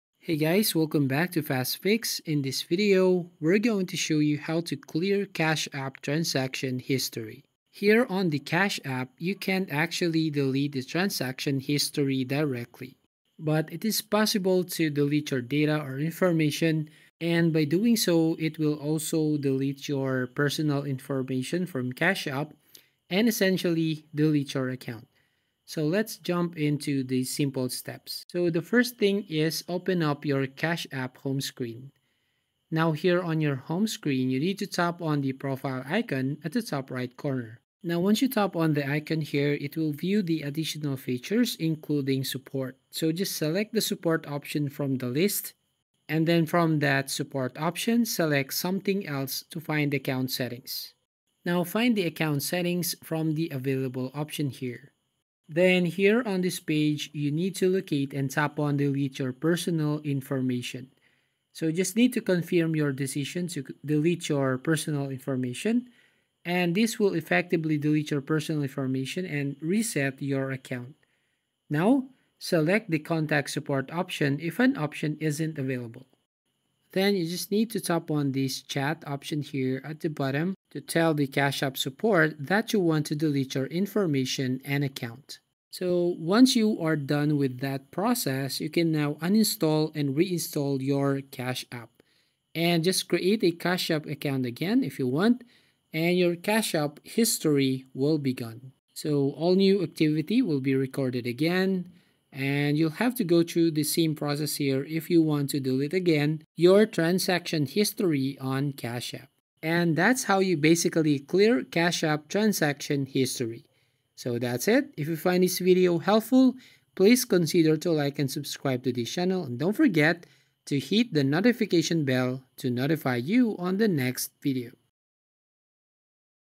How to Clear CASH APP Transaction History (Easy Way)
In this video I'll show you How to Clear Cash App transaction history (Easy Way). The method is very simple and clearly described in the video. Follow all of the steps & you will know how to clear transaction history on cash app.